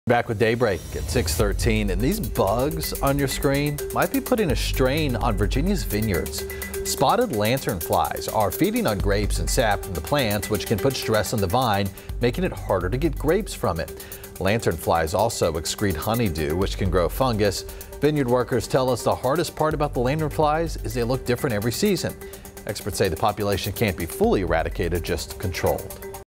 This invasive bug could be threatening Virginia vineyards
The spotted lanternfly looks different every season, and it puts stress on vines that can impact grape production.